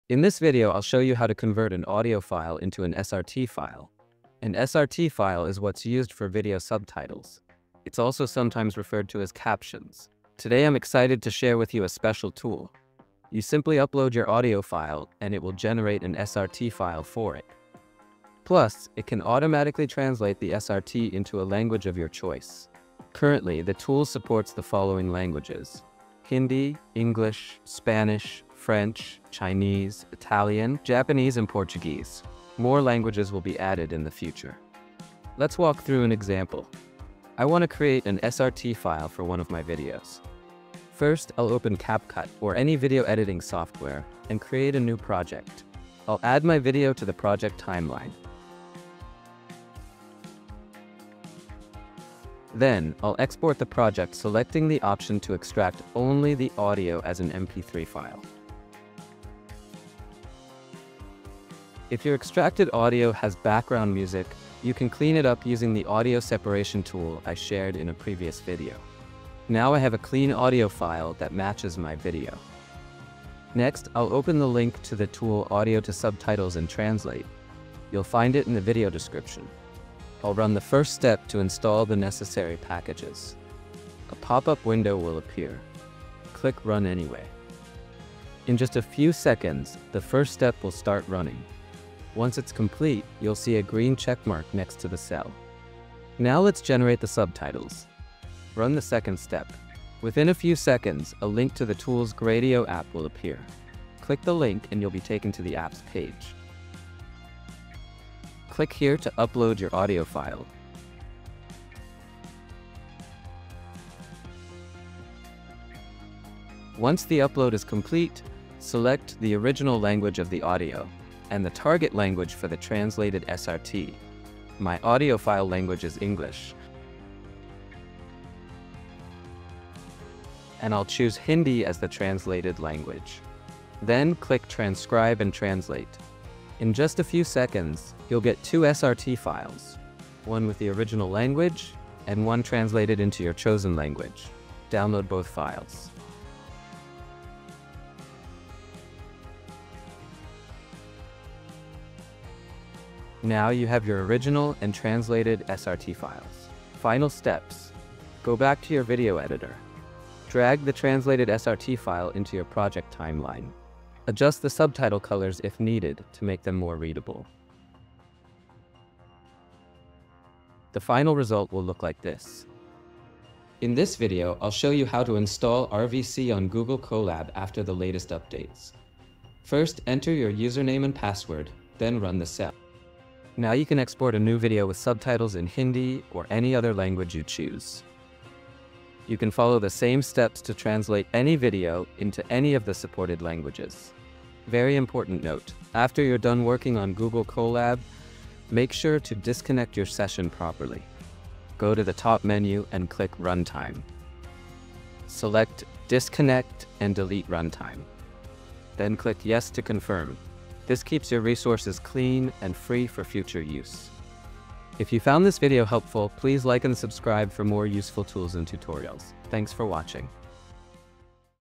How to Convert Audio to SRT Subtitles and Translate Them with One Click!
In this video, I'll show you how to easily convert any audio file into an SRT subtitle file — and even translate your subtitles into another language!

Whether you're a content creator, editor, or just want to add professional subtitles to your videos, this tutorial will walk you through the full process step-by-step using a free tool.

🌍 Supported languages: Hindi, English, Spanish, French, Chinese, Italian, Japanese, Portuguese.

Watch till the end to learn how to extract clean audio, generate subtitles, and translate them for global reach!

✅ This channel "Bahaa AI" specializes in publishing tutorial for artificial intelligence tools to create videos, photos and music using a mobile phone.